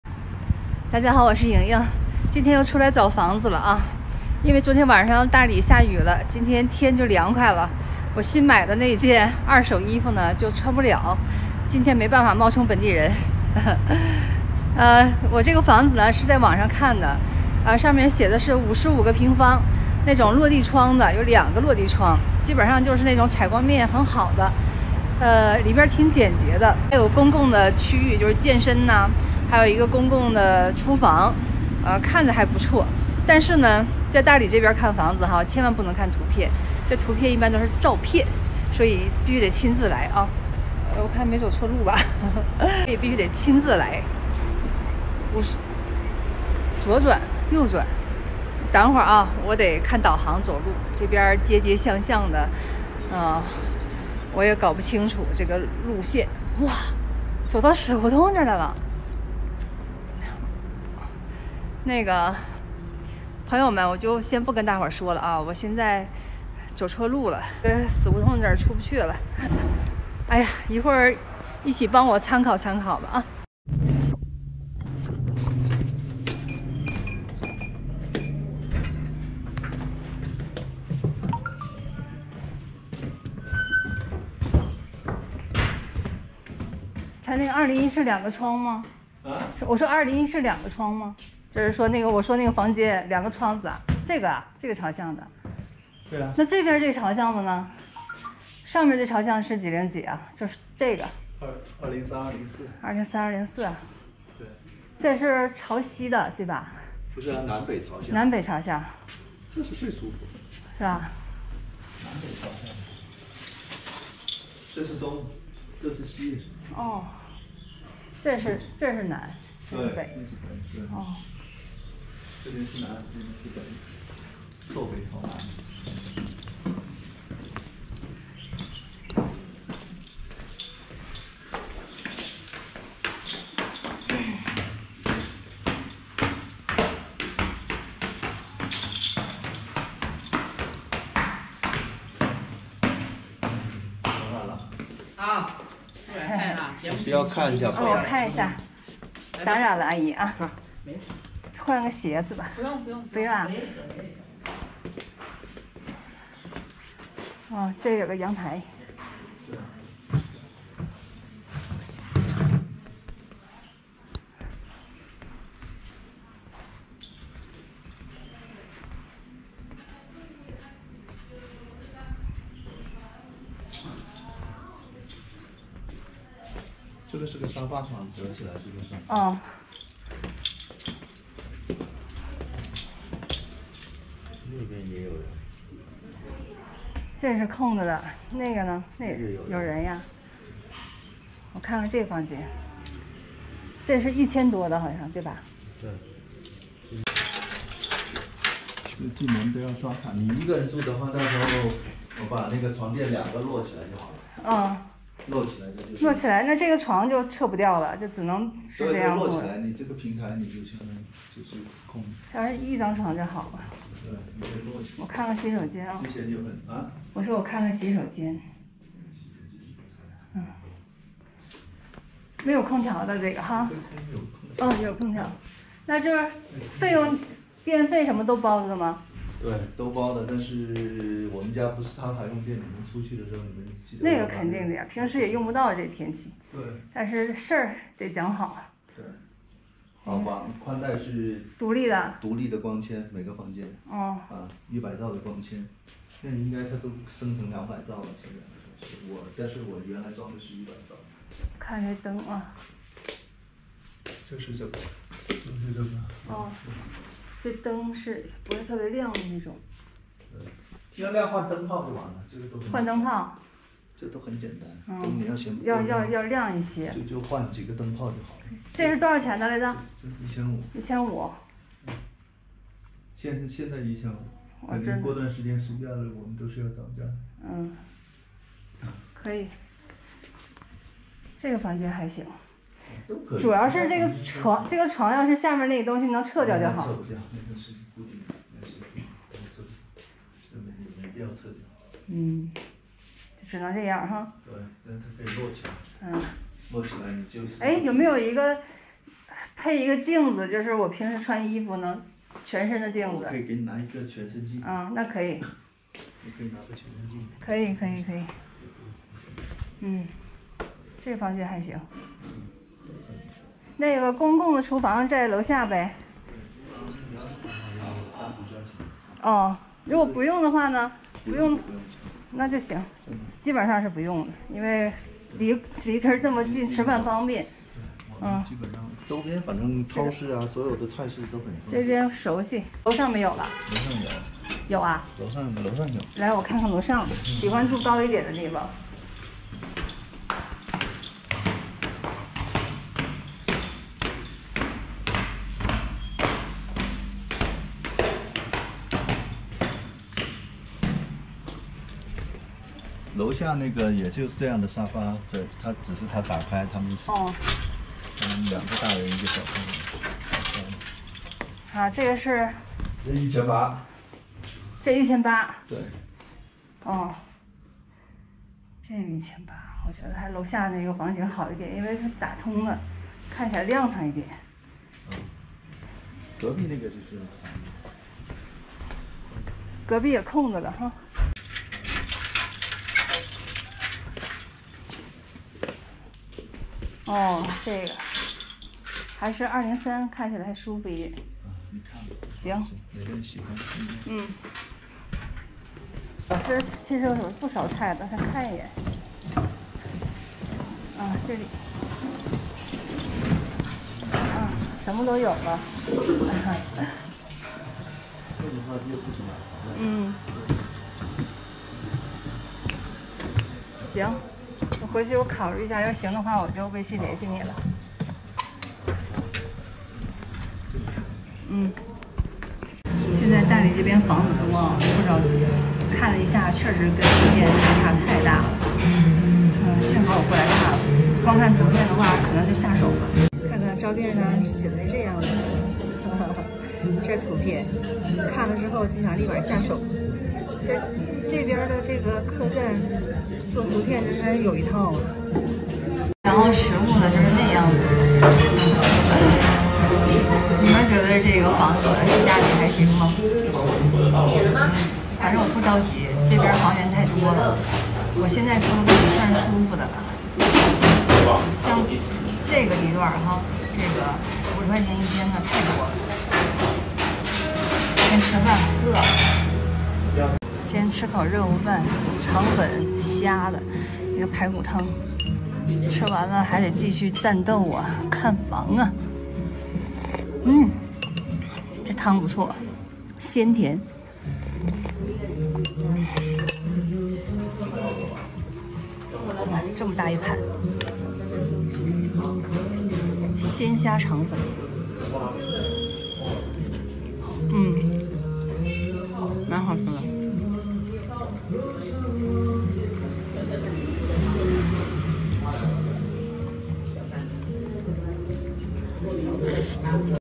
美女在大理长租房子，55平方落地窗送阳台2100一个月，这房子值吗？【莹莹】
莹莹每天与大家分享东北生活日常见闻和美食搜罗视频